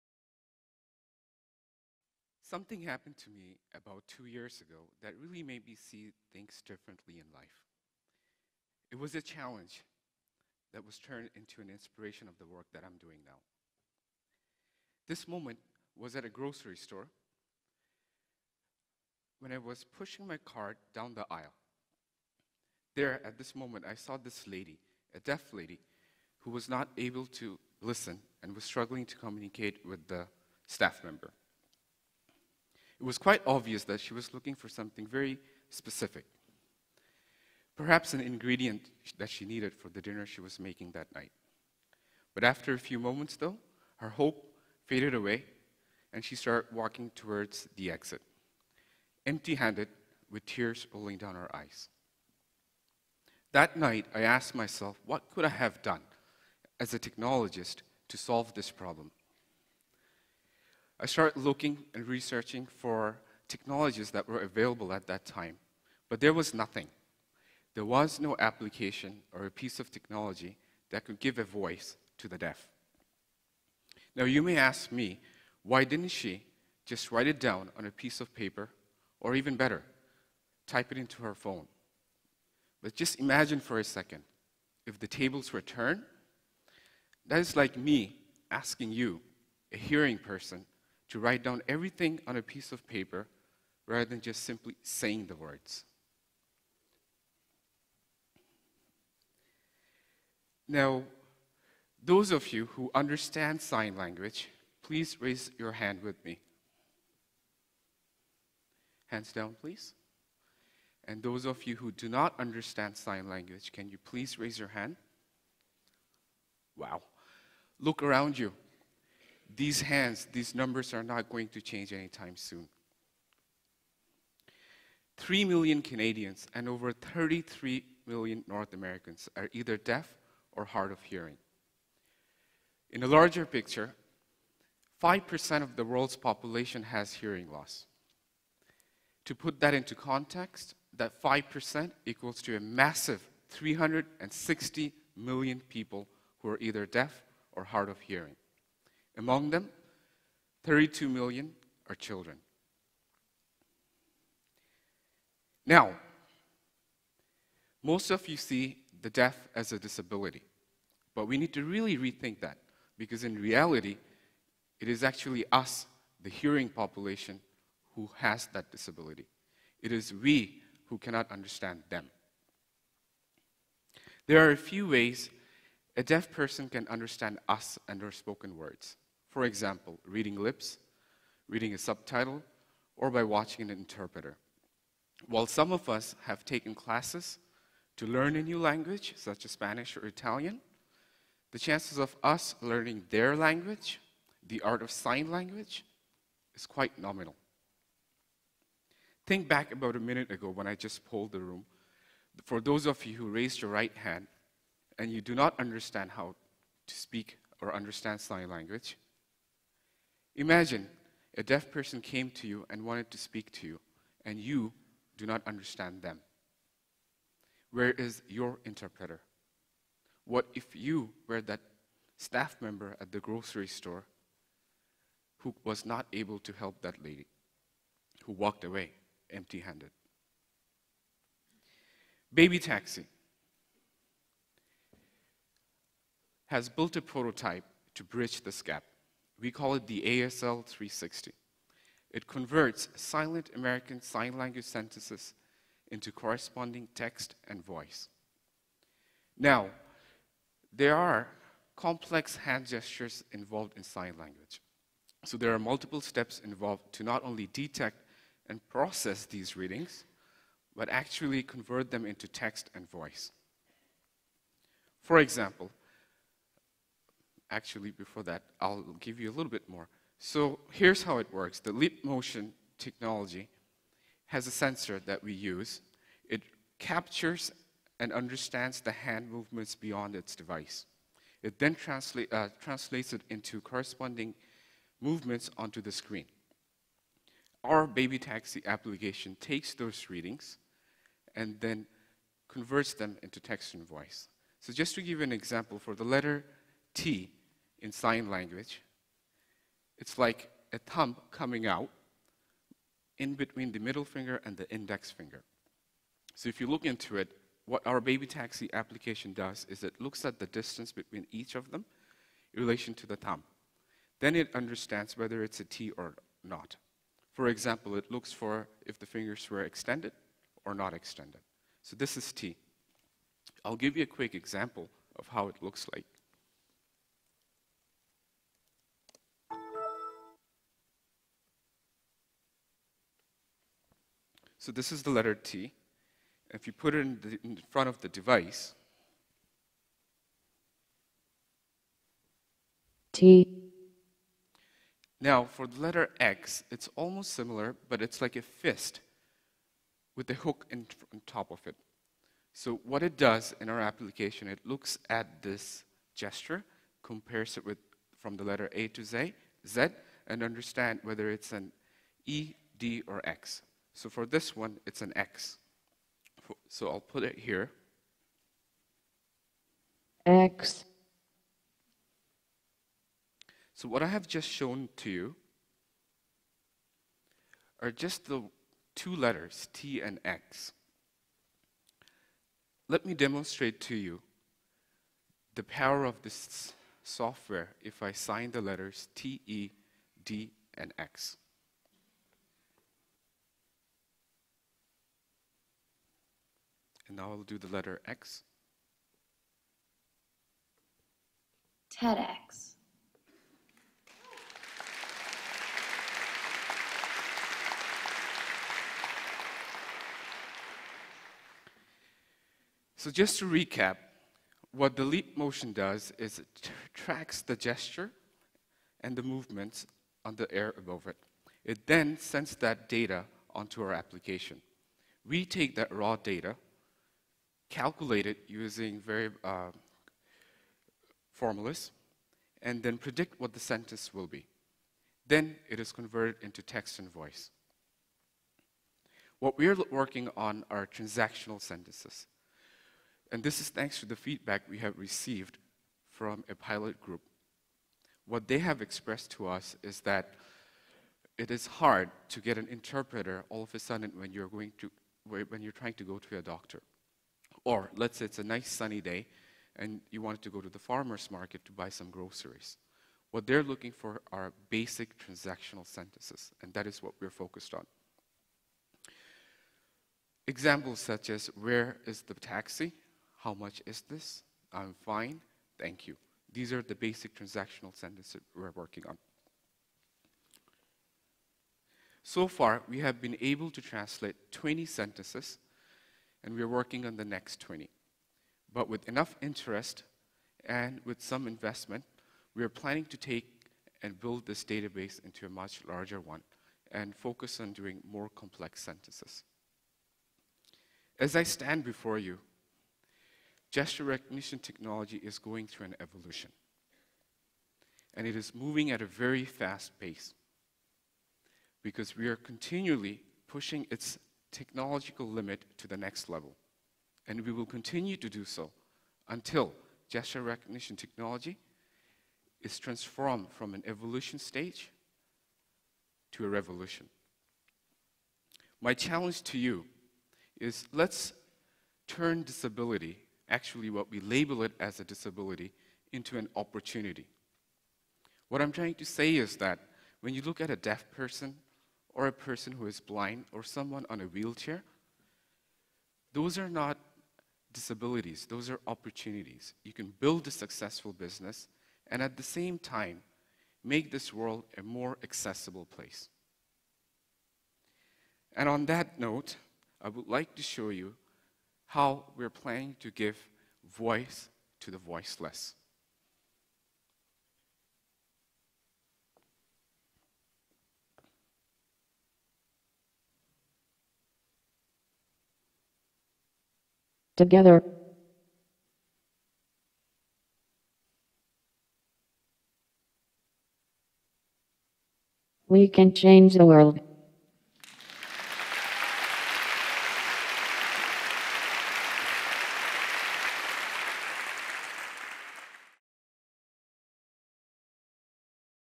Giving a Voice to the Voiceless | Tarik Sayeed | TEDxToronto
Tarik Sayeed and his company BabyTaxi believe the latest advances in gesture sensing technology have the ability to give a voice to the voiceless, and change the way people think about disabilities.

Tarik Sayeed has held several leadership roles in IT with companies such as JumpTV, Comcast, and First West Credit Union. He was nominated for IT Manager of the Year in 2011. He left a successful career to launch BabyTaxi, a tech start-up company building software using gesture recognition technology to translate American Sign Language into corresponding text & voice. Tarik studied at NJIT, the University of Waterloo, and has a leadership certification from MIT.